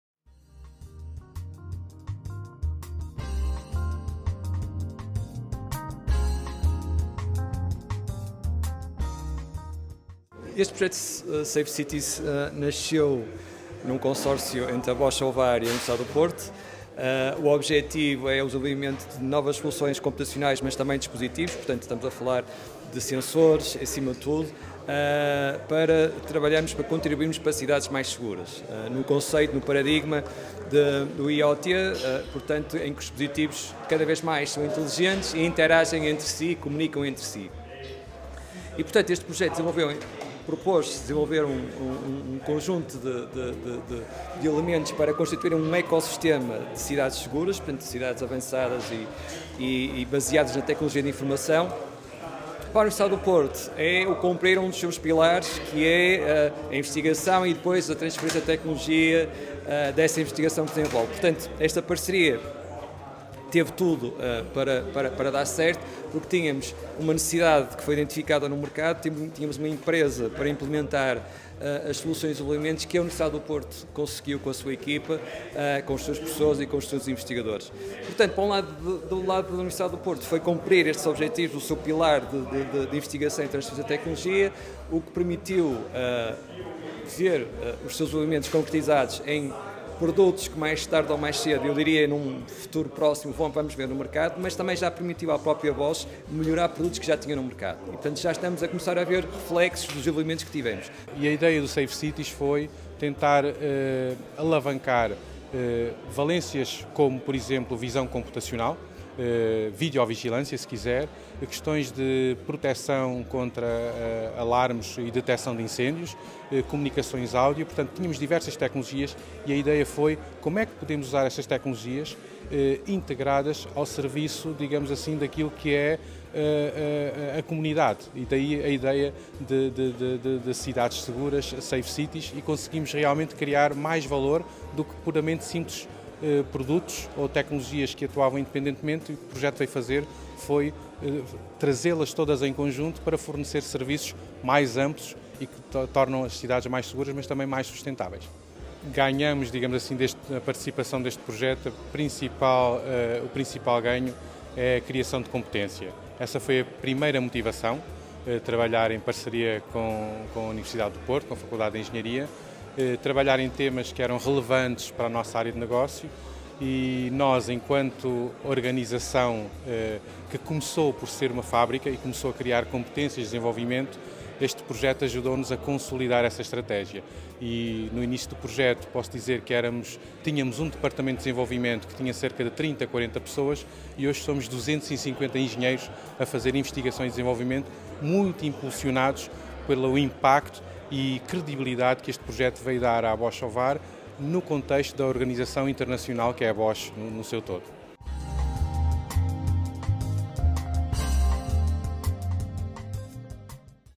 Apresentação final de resultados do projeto “Safe Cities” na FEUP
A apresentação dos resultados de projeto de inovação “Safe Cities”, com um orçamento de 16 milhões é um consórcio realizado entre a Universidade do Porto e a Bosch, e decorreu no passado dia 13 de dezembro na Faculdade de Engenharia da U.Porto.